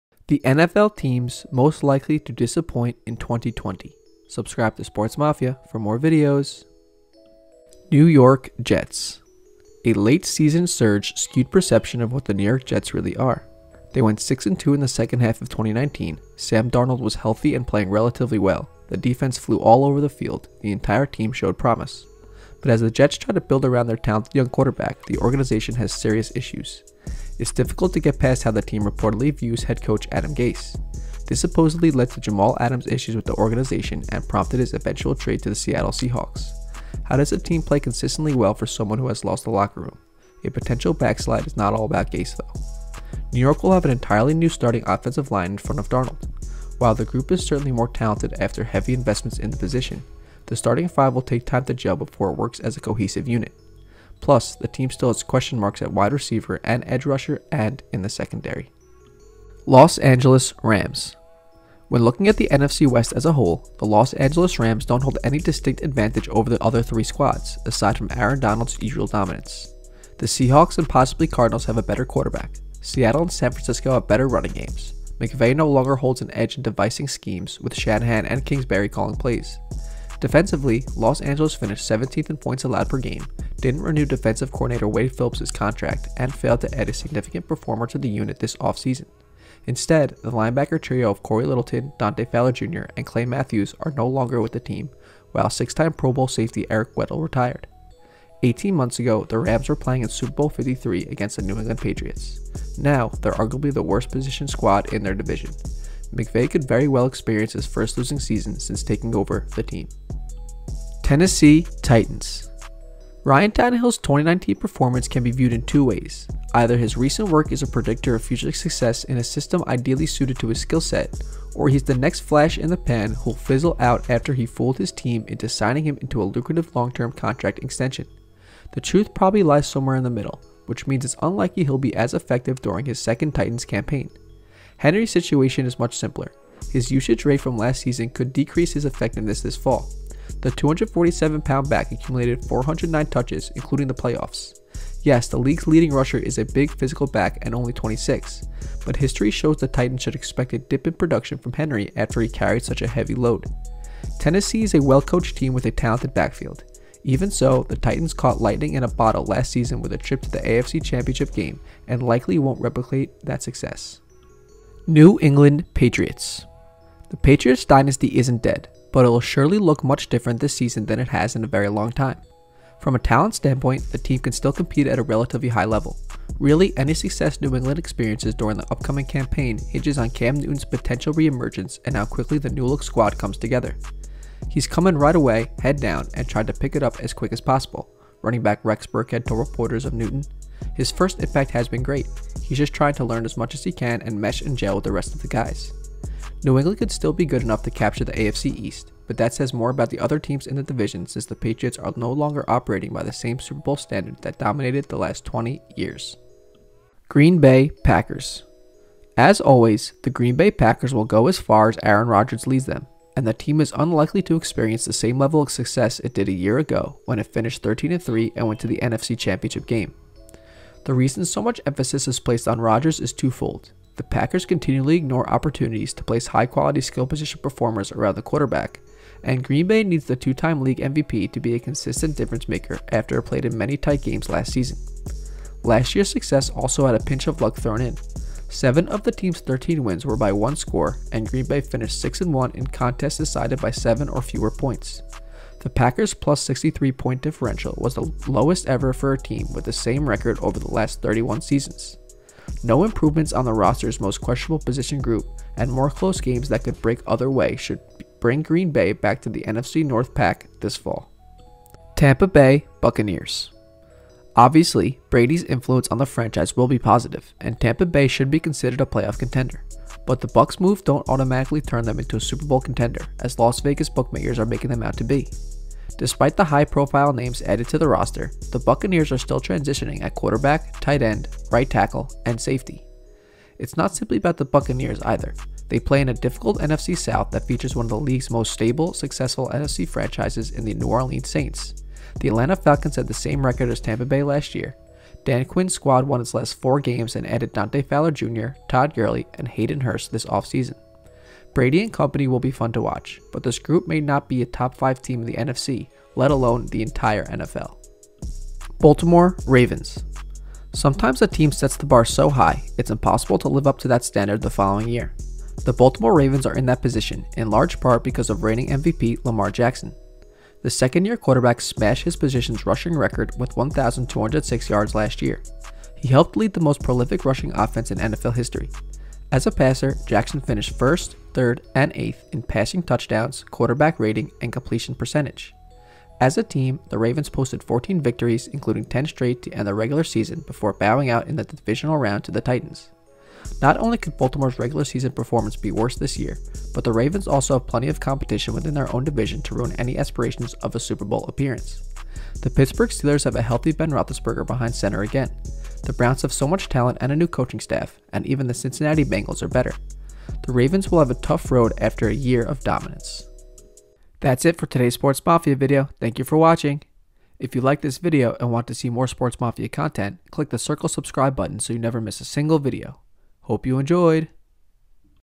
NFL: The Teams Most Likely to Disappoint in 2020 (Ravens, Rams, Buccaneers)
In today's Sportsmafia video, we discuss the NFL that are most likely to have a disappointing season in 2020.

Some of the featured teams are:

Tennessee Titans
Baltimore Ravens

Sportsmafia is the #1 channel for all of your sports news and stories!!